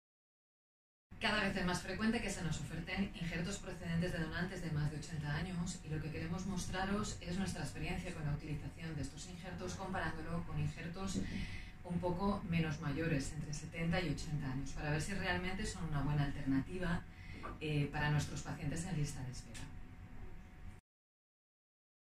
Donantes con edad ≥80 años ¿son una buena alternativa para nuestros pacientes en lista de espera?
A. Sancho, E. Gavela, J. Kanter, A Ávila, S.Baldoví, S Beltrán , María, C.Castro, LM Pallardó. Servicio de Nefrología Hospital Universitario Dr Peset, Valencia.